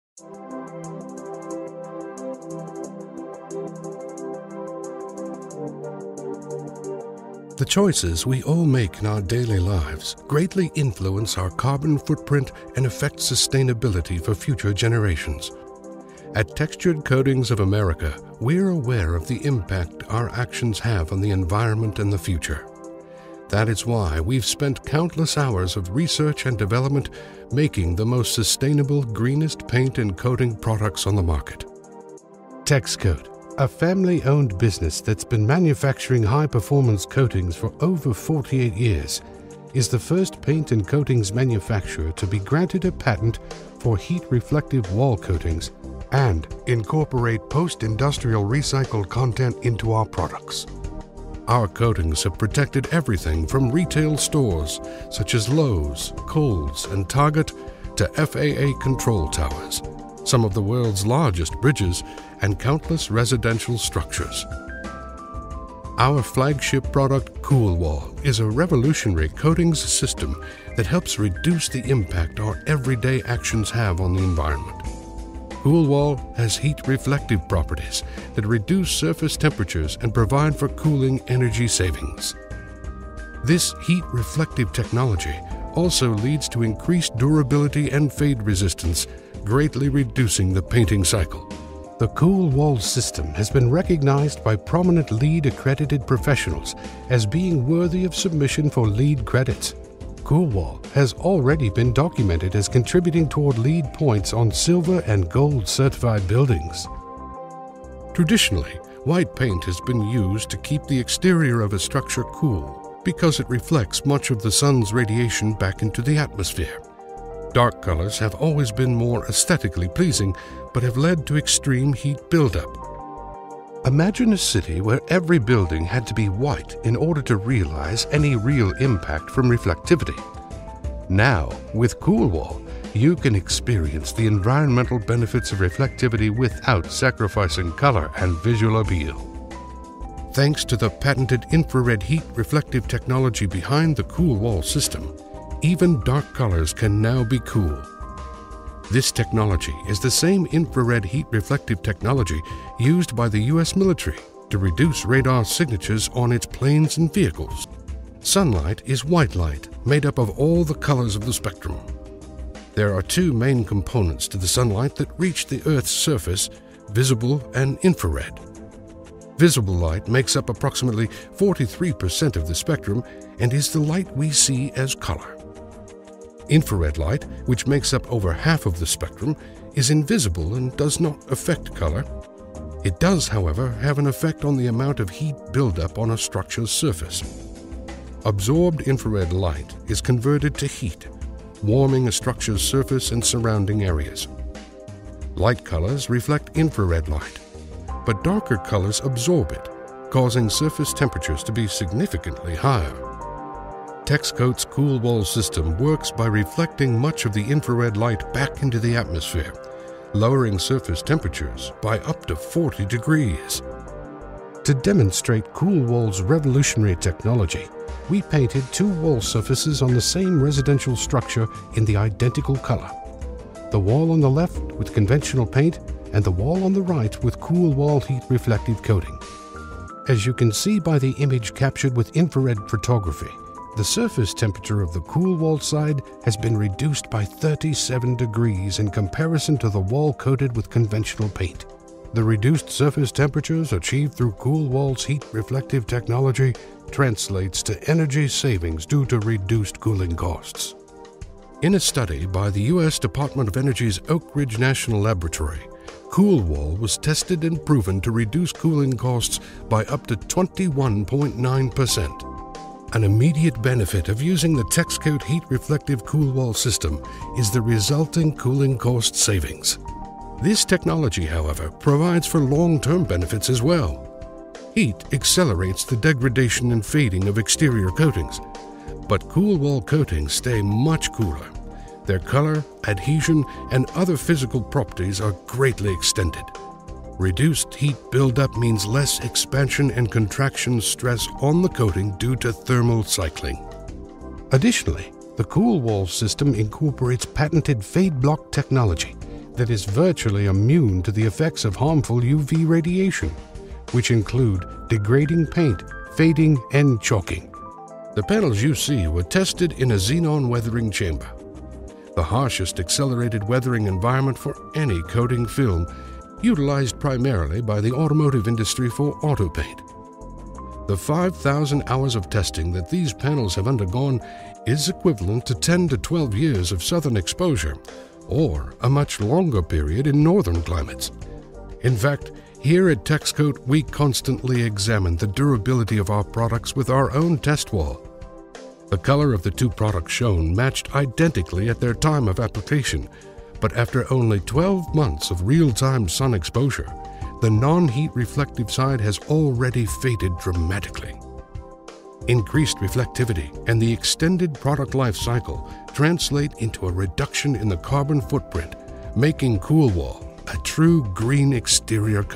COOLWALL® Infrared (IR) Heat-Reflective Exterior Wall Coating System
TEX-COTE®'s COOLWALL® Infrared (IR) Heat-Reflective Exterior Wall Coating System is a revolutionary coating system that has been tested by the U.S. Department of Energy and  proven to reduce surface temperatures and provide cooling cost energy savings.